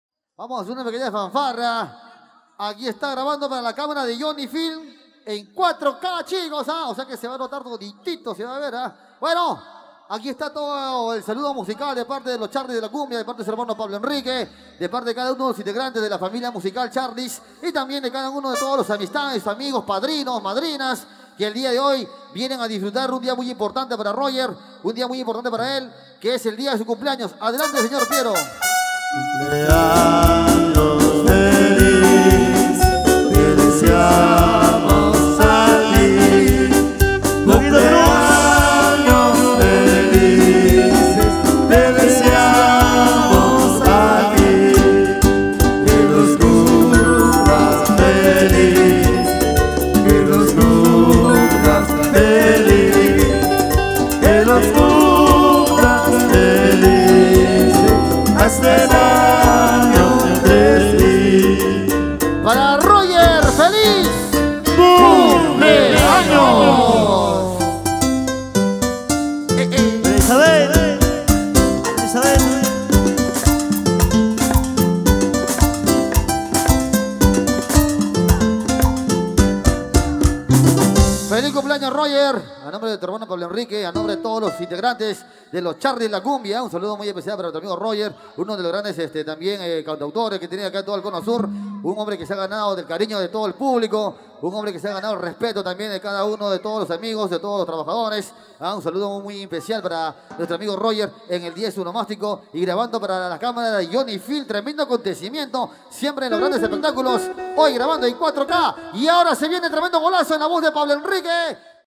FELIZ CUMPLEAÑOS ROGER...(D.R.) LOS CHARLIS DE LA CUMBIA 2020 en 4K
FELIZ CUMPLEAÑOS ROGER DE LOS CHARLIS DE LA CUMBIA
AGRADECIMIENTO ESPECIAL A "MIA PRODUCCIONES"
EVENTO REALIZADO: SABADO 07 DE MARZO DEL 2020 / ENTRADA DE VILLA EL SALVADOR
GRABADO GRACIAS A JHONNYFILMS: CONTACTOS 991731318